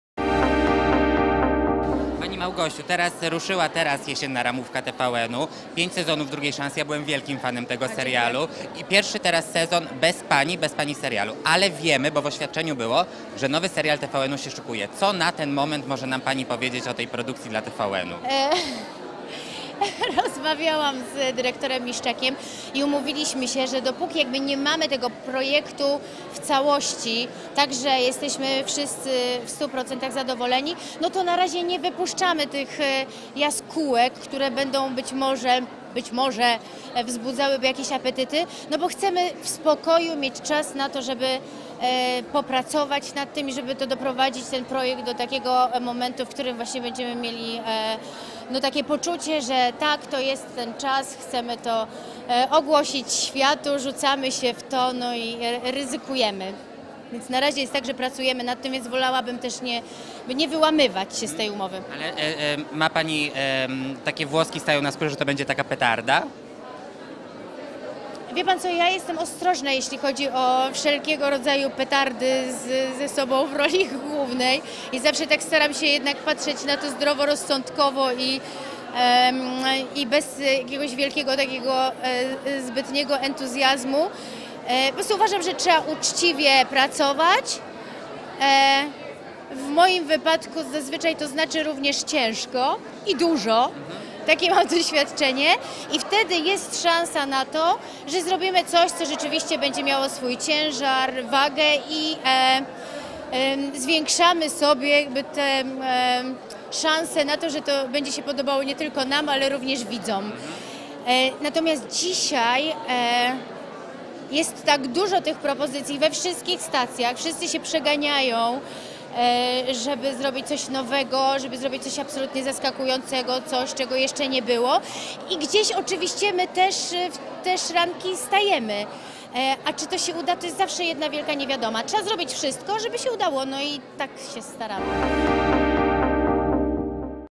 Małgorzata Kożuchowska o nowym serialu TVN z jej udziałem
W najnowszej ramówce TVN pierwszy raz od wielu sezonów nie pojawiła się żadna produkcja, w której występuje Małgorzata Kożuchowska. W maju bowiem stacja zakończyła emisję popularnego serialu „Druga szansa”, który na antenie TVN doczekał się aż pięciu serii. Jednak w oświadczeniu o zakończeniu produkcji, stacja zaznaczyła, że już niedługo na ich antenie pojawi się nowy serial z Kożuchowską w roli głównej. Co to będzie za produkcja? Zapytaliśmy u źródła!